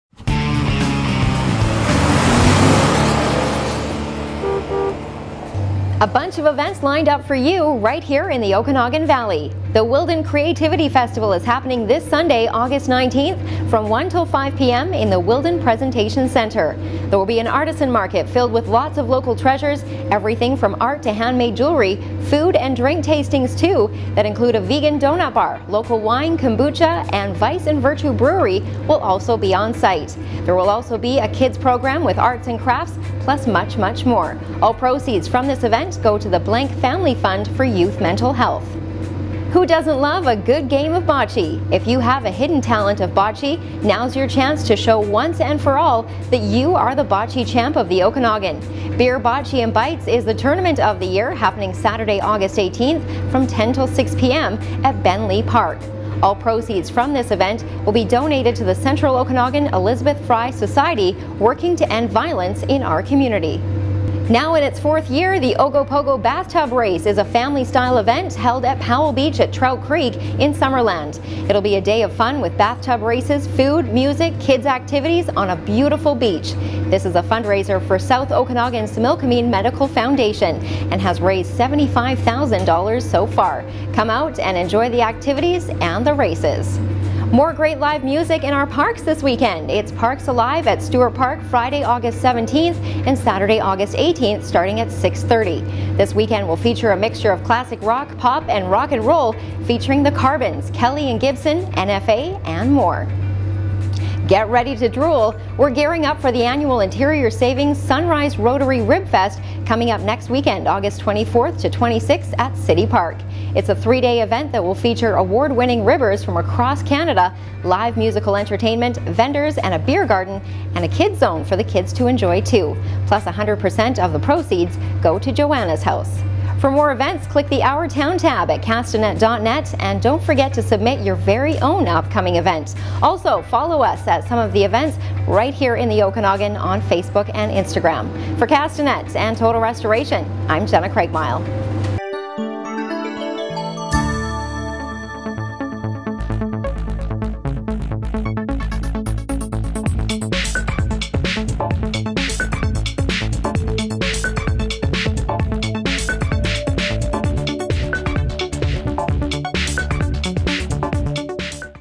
Weekend full of fun
Check out what's happening this weekend with Jenna Craigmyle, who'll be making the rounds in the Castanet-Total Restoration Total Fun Cruiser.